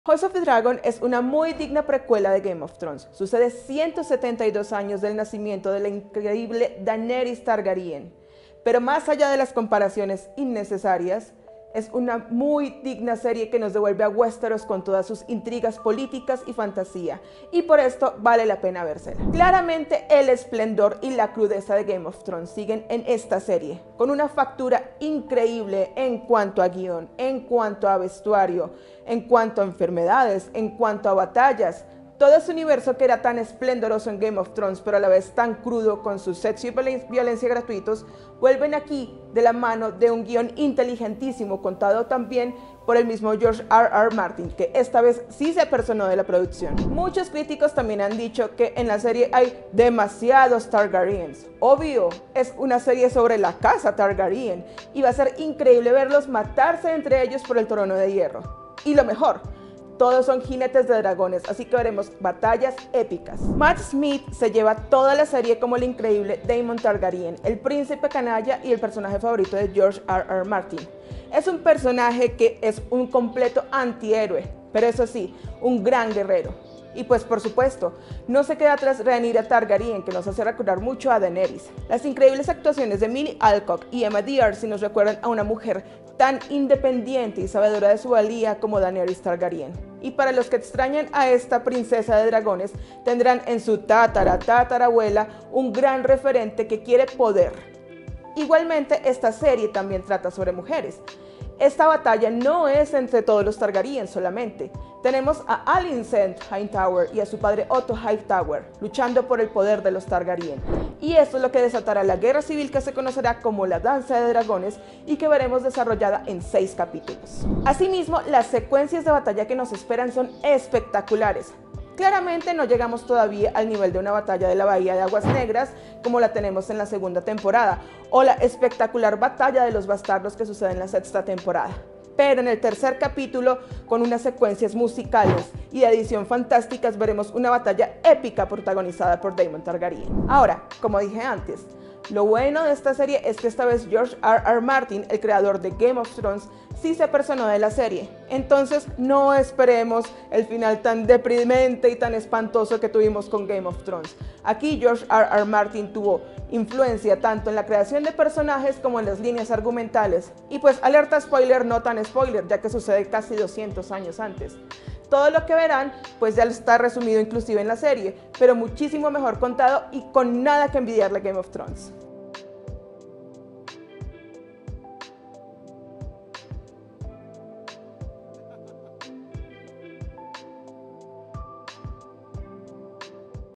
Review House of the Dragon: 5 puntos clave
House of The Dragon es una muy digna precuela de Game of Thrones, con su espectacular producción y con una historia que bueno, alerta spoiler (que ni spoiler) sucede 172 años antes del nacimiento de la increíble Daenerys Targaryen.